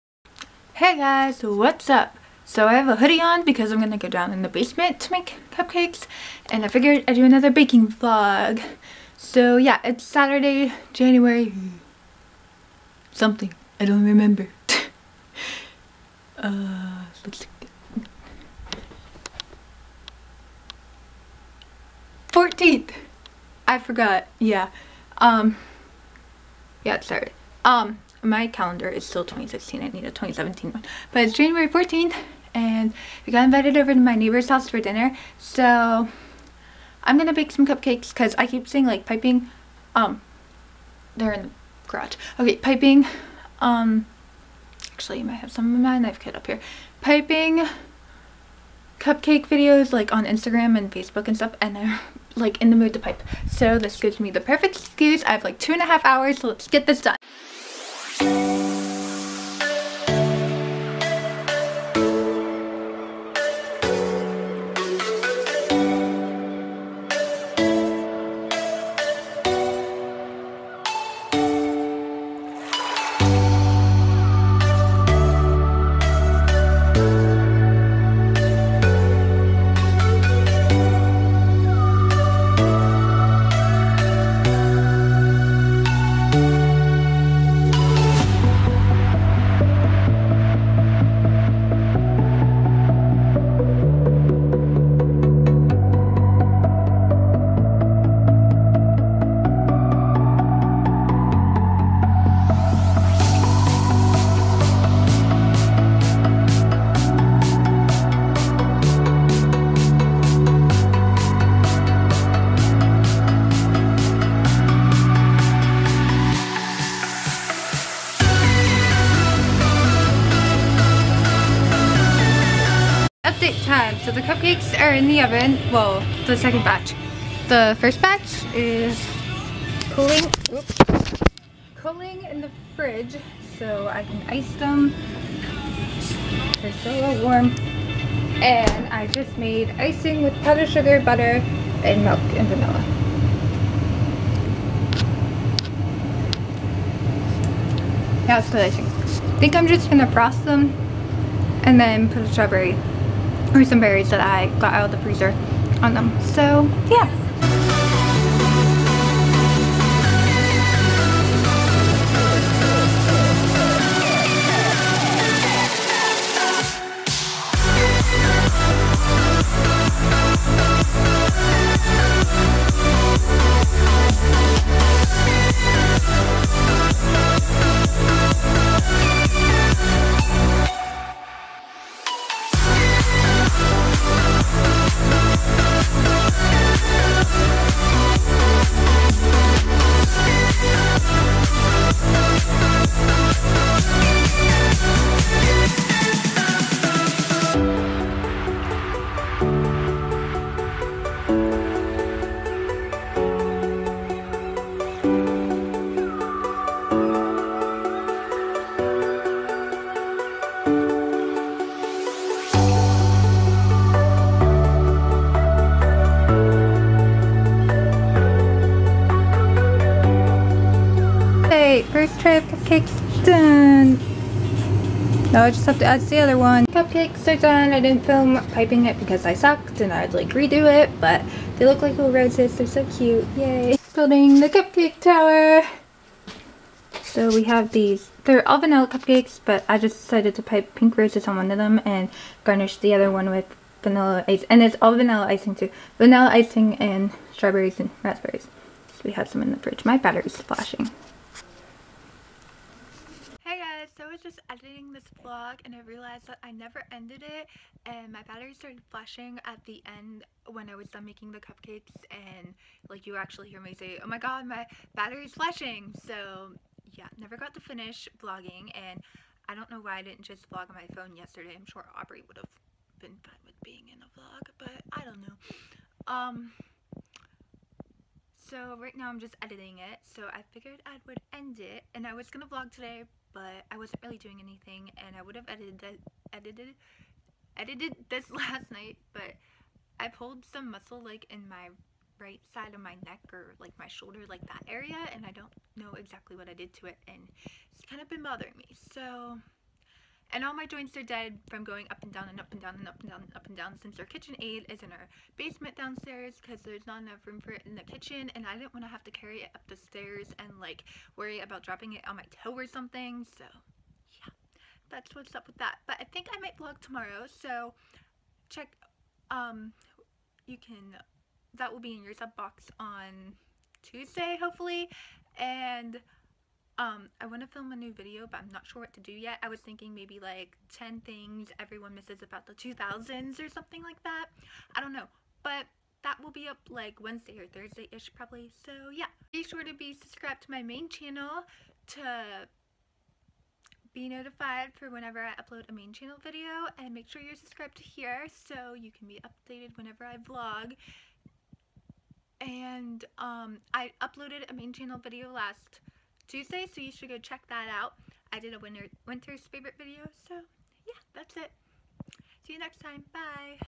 Baking Strawberry Cupcakes!
I made cupcakes for dessert yesterday and figured I vlog. Don't mind my basement I didn't want to drop the mixture on the stairs on my toe or something.